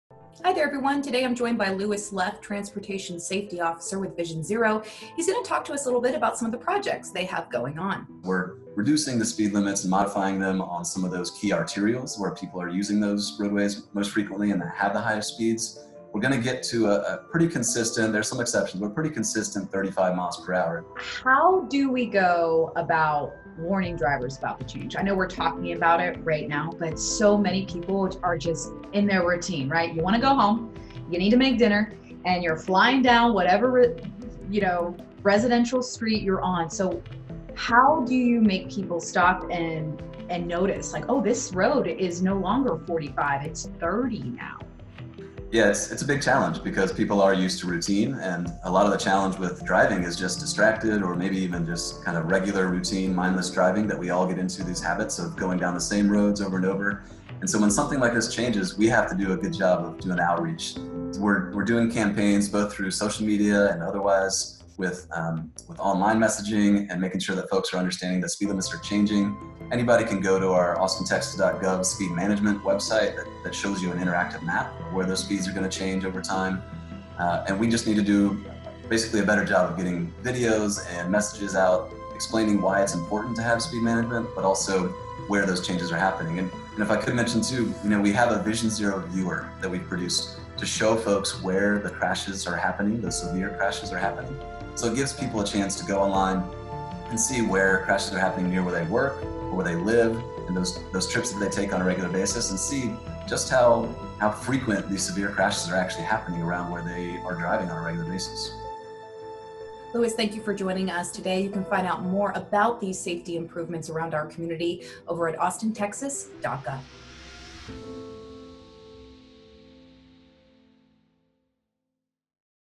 Traffic Q&A: Speed Limit Changes Part 2
Amanda Dugan speaks with Austin's Transportation Safety Officer about speed limit changes in Austin.